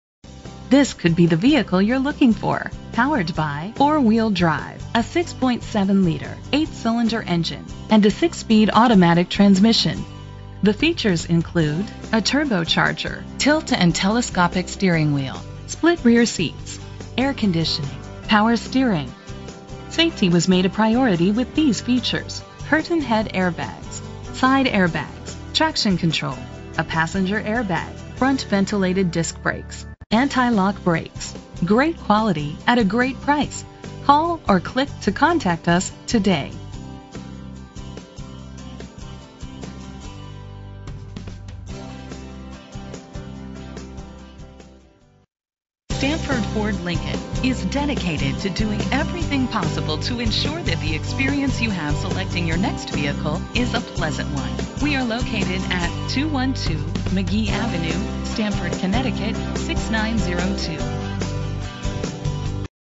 2015 Ford F-450 Chassis Cab - Stamford CT
Phone:
Year: 2015
Make: Ford
Model: F-450 Chassis Cab
Trim:
Engine: 6.7 liter 8 cylinder 32 valve
Transmission: 6-Speed Automatic
Color: Silver
Mileage: 10
Address:
212 Magee Avenue